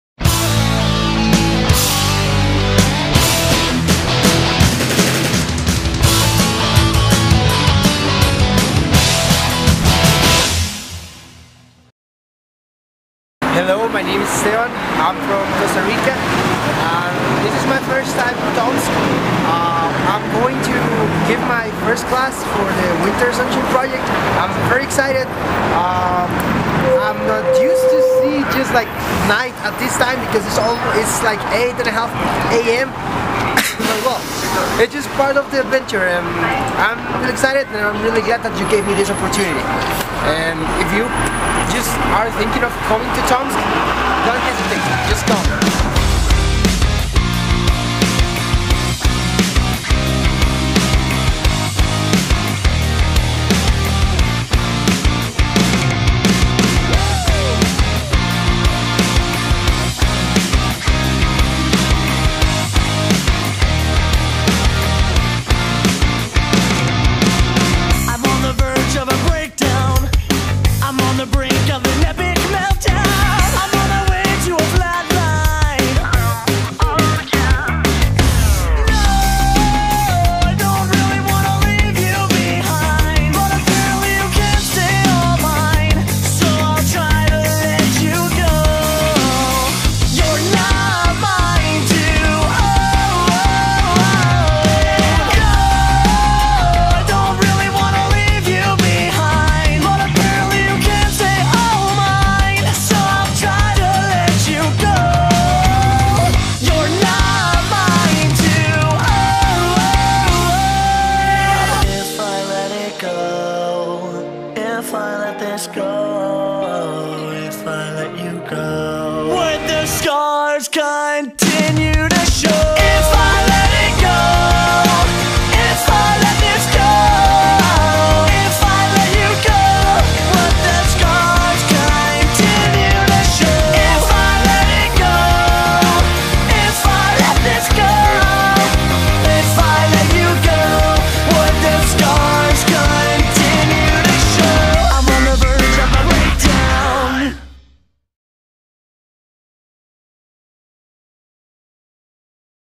Esteban Marin Corrales
ЭстебанМарин Корралес и его стажировка AIESEC в городе Томске